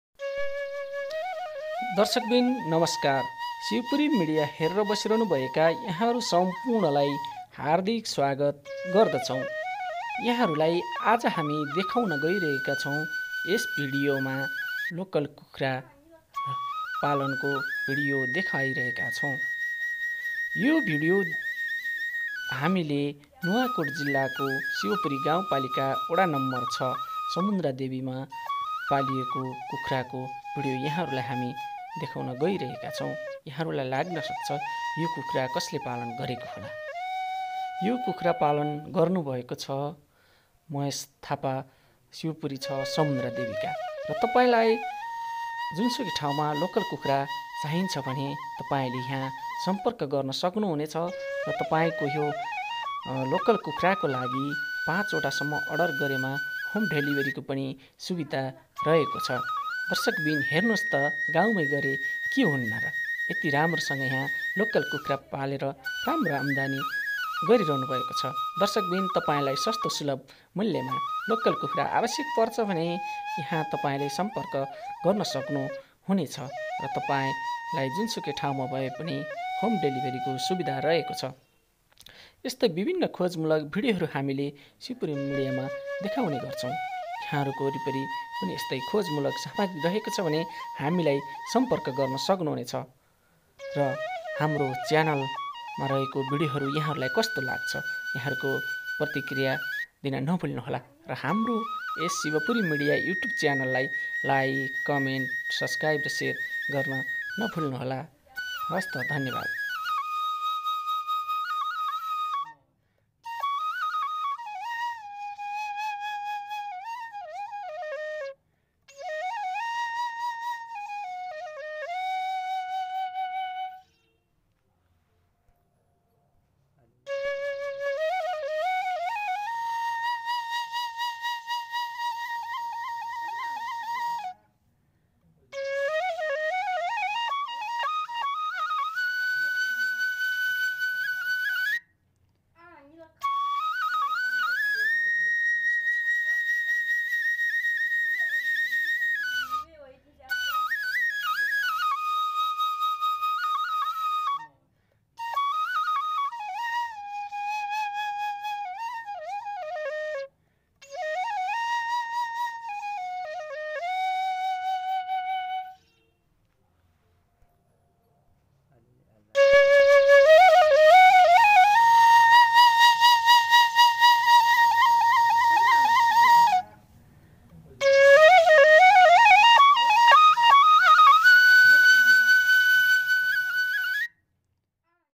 एक कल फोन गर्नुहोस सस्तोमा शिवपुरीको लोकल कुखुरा खानुहोस
यश भिडियोमा हामीले नुवाकोट जिल्लाको शिवपुरी गाउँपालिका वडा न ६ मा रहेको ल‍ोकल कुखुरा भिडियो राखेका छौ । तपाईंहरुलाई अावश्यक छ भने यश नम्बर मा सम्पर्क गर्नुहोस। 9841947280/